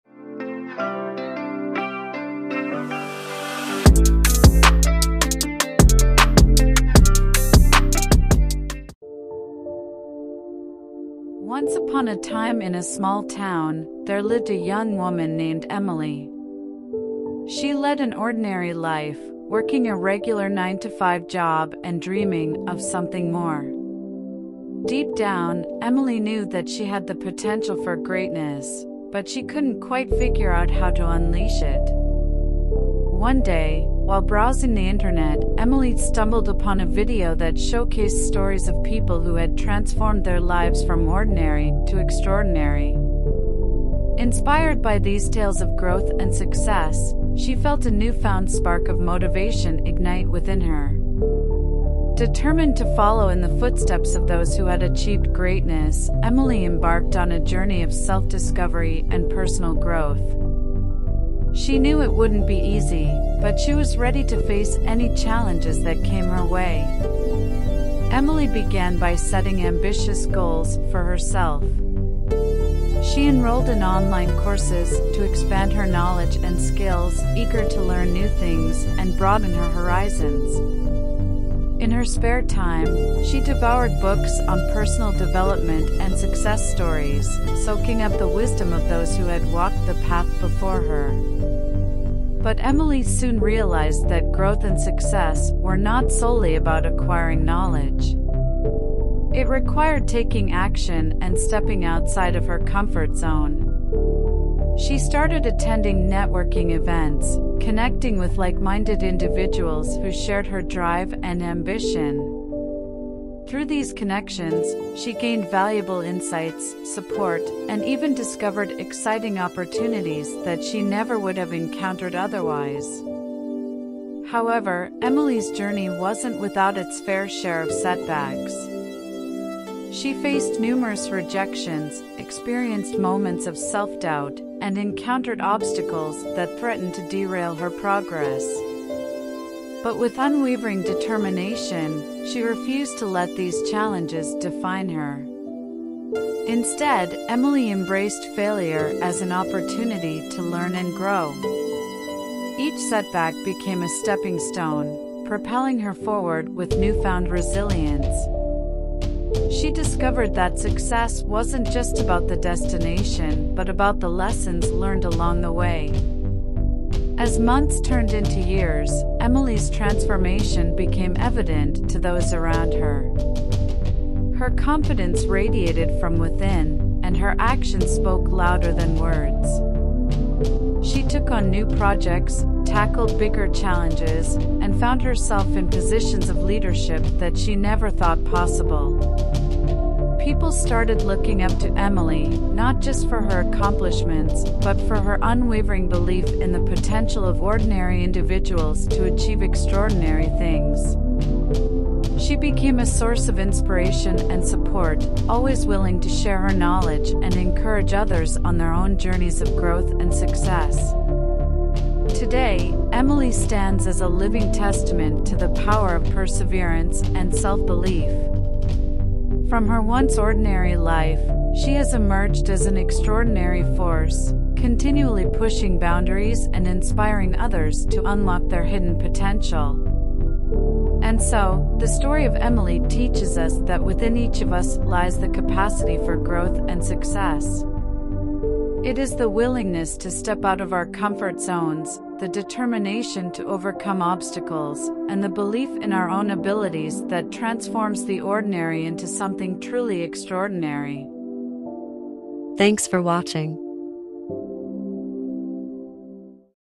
Successful lady | A short story 2023
"From Ordinary to Extraordinary: Inspiring Stories of Growth and Success" is a captivating tale that follows the journey of Emily, an ordinary young woman who yearns for more in life. Motivated by stories of transformation, she embarks on a path of self-discovery, setting ambitious goals, acquiring knowledge, and connecting with like-minded individuals. Despite facing setbacks and doubts, Emily's unwavering determination leads her to triumph over challenges and embrace failure as a stepping stone to growth. Through her remarkable transformation, she becomes an inspiration to others, showcasing the power of perseverance and self-belief. This story reminds us that within each of us lies the potential for greatness and that the journey from ordinary to extraordinary is within reach for those who dare to pursue it.